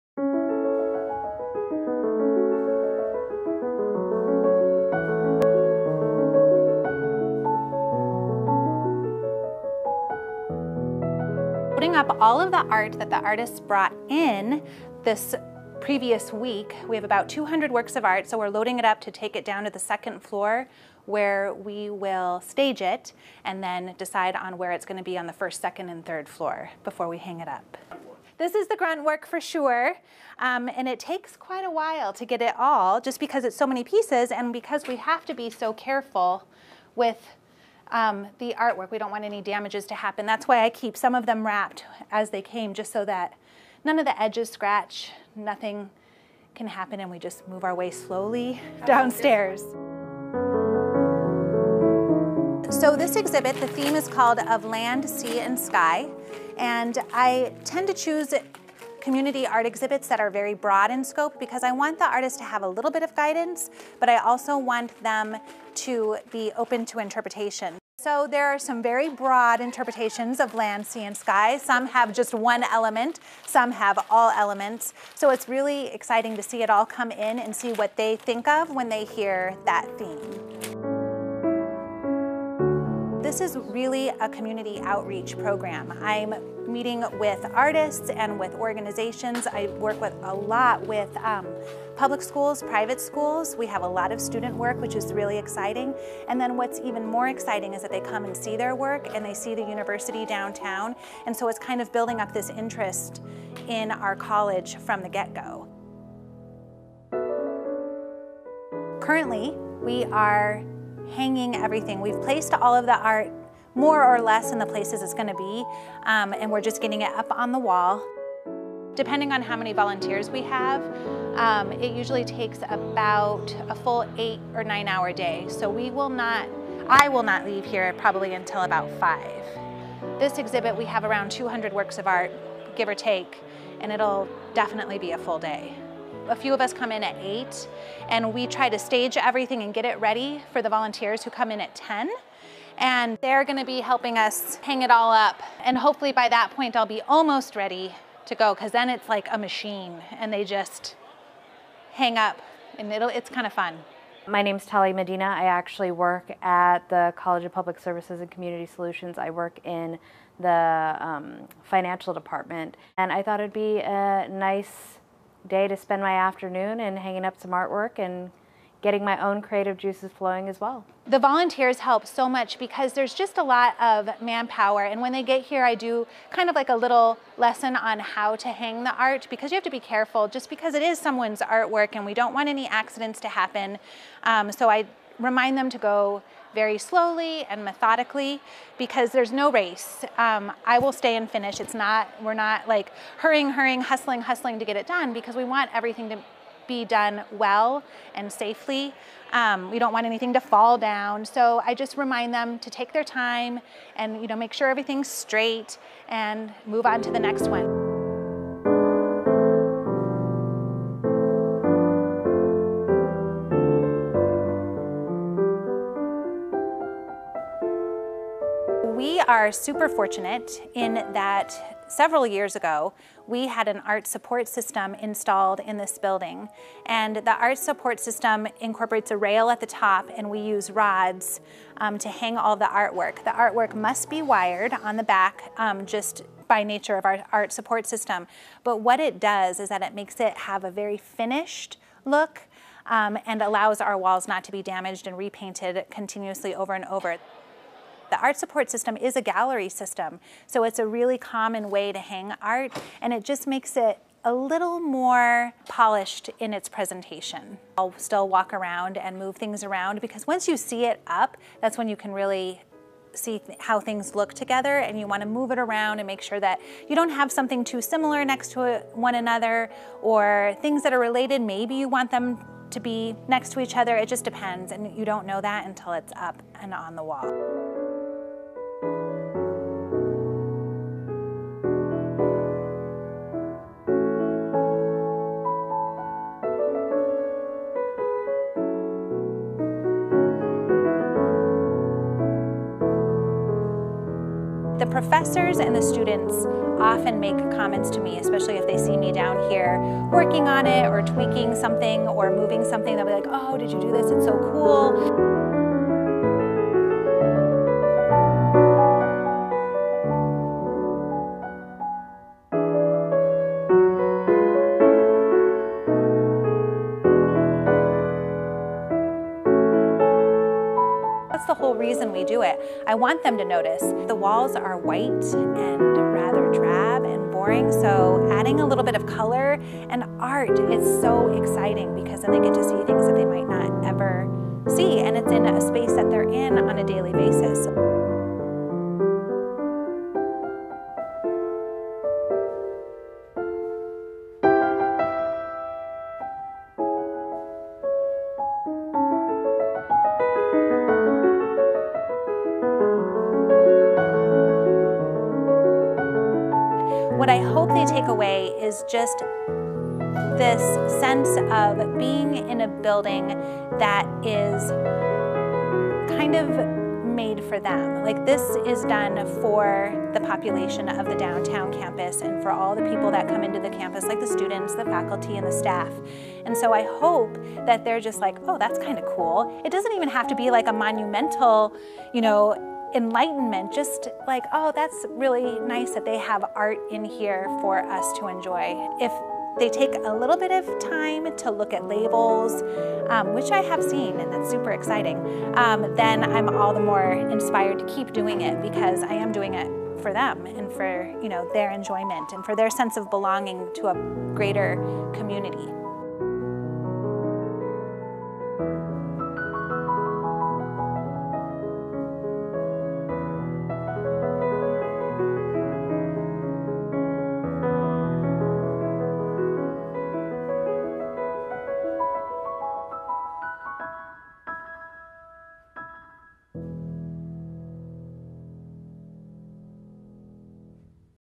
Art as Public Service: ASU Art Exhibit Features Community Creations
A look at the ASU College of Public Service and Community Solutions' Fall 2016 exhibit: "Of Land, Sea, and Sky." Each year, the College curates an exhibit of community art that is displayed throughout the University Center building on ASU's Downtown Phoenix Campus. These semester-long exhibits show the value of art as a community service.